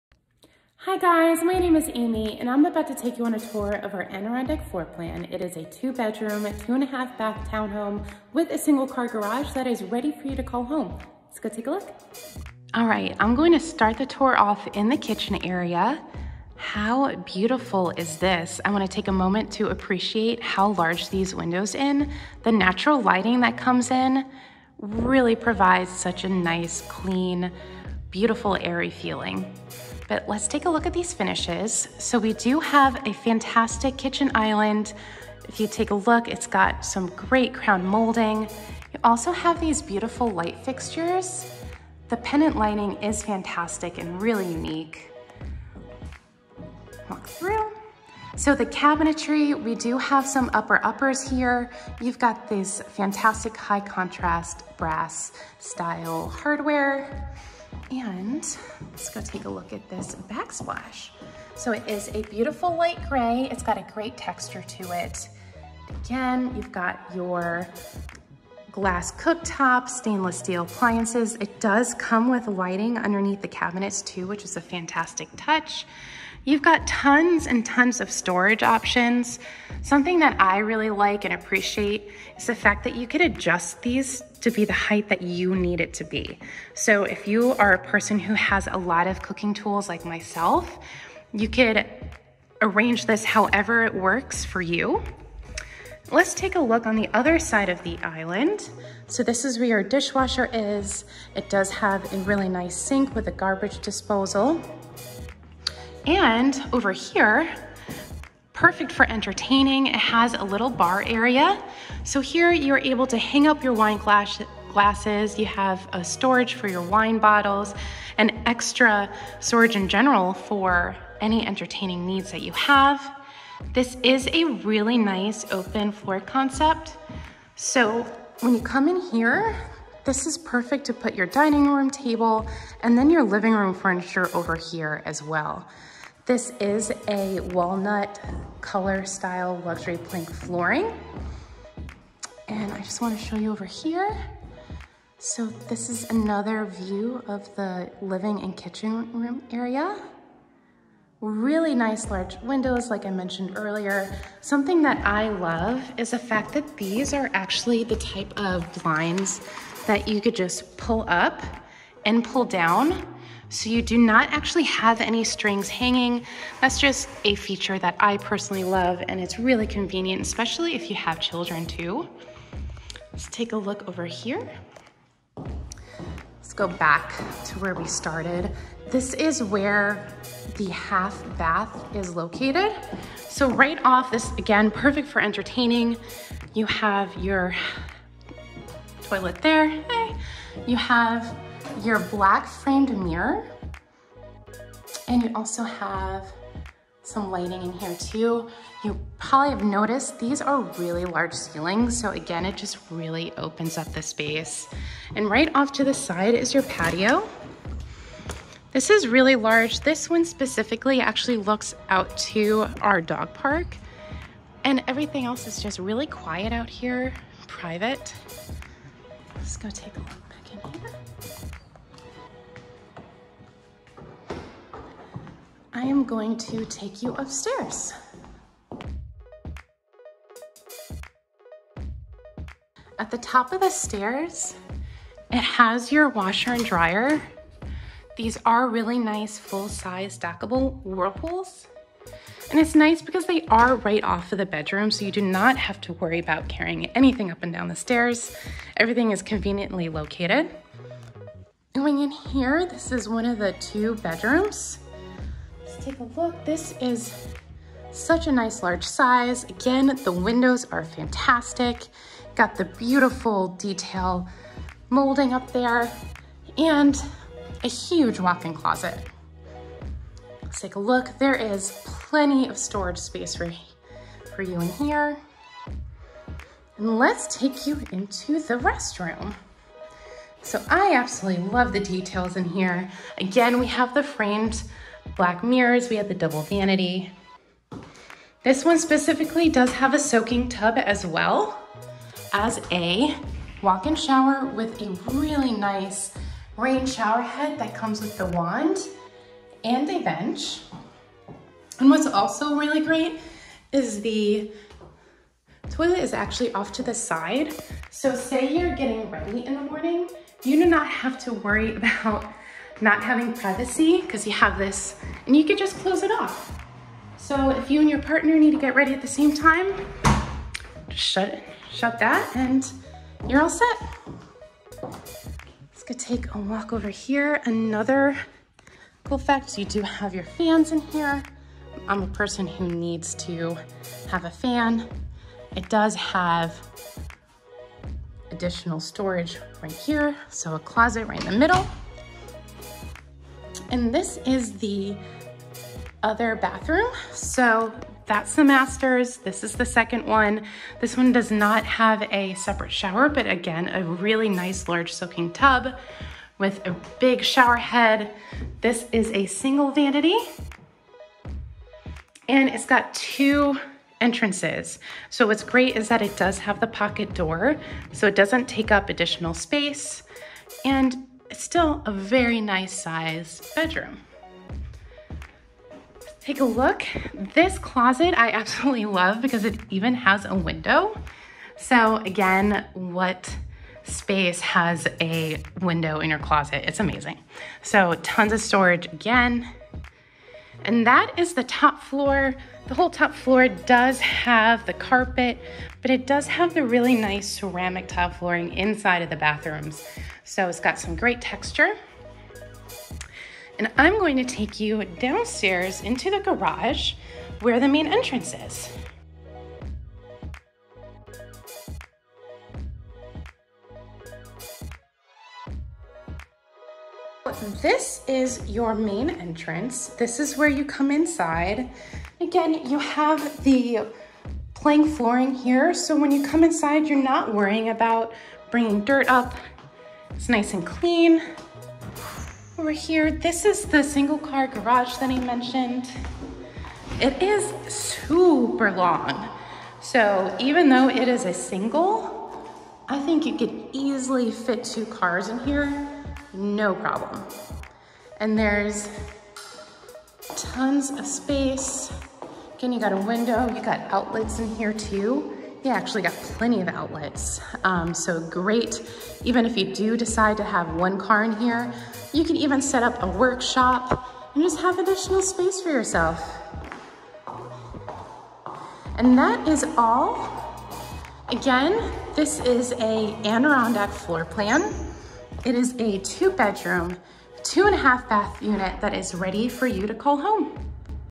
Adirondack Floorplan (2,367 sq ft) 2 Bed / 2.5 Bath With 2-Car Garage - Townhomes at Bridlestone
The Townhomes at Bridlestone is a luxury townhome community located in South Charlotte’s most desirable suburb, Pineville, and two miles from the affluent Ballantyne neighborhood. Our two-bedroom and three-bedroom townhomes for rent are designed to delight. With impressive finishes, private attached garages, and spacious patios/balconies, our units make it possible to live beyond your expectations.